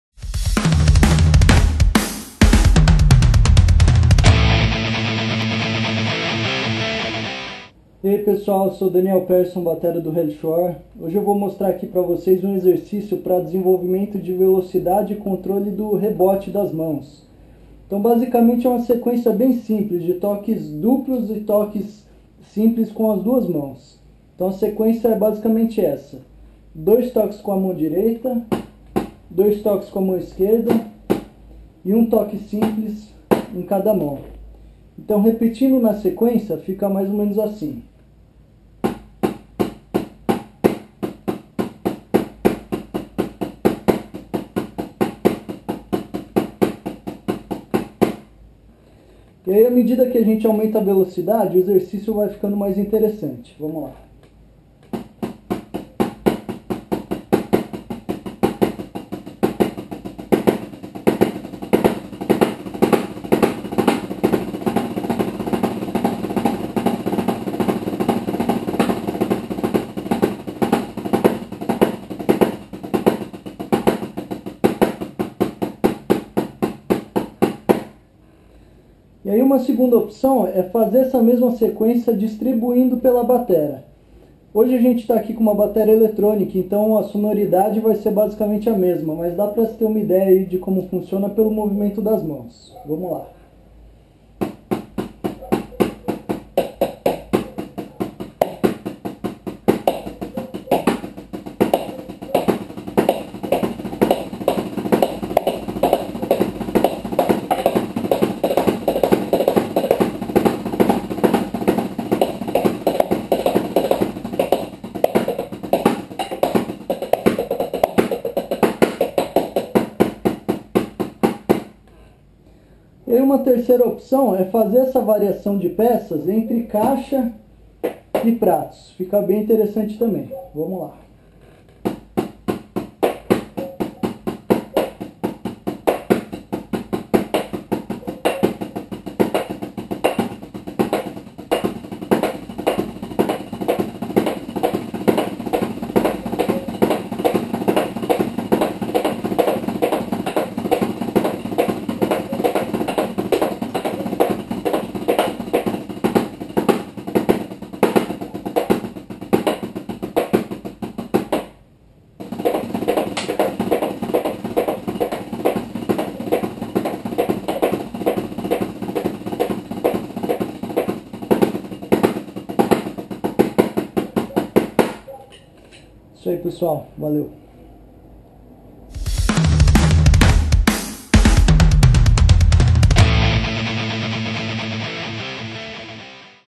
Drumming Exercise (Six Stroke rolls) - Speeding Hands Technique
Six Stroke Roll (paradiddle-diddle) - Hands exercise to develop speed and rebounds
Sequence: R R L L R L
R: Right Hand
L: Lef Hand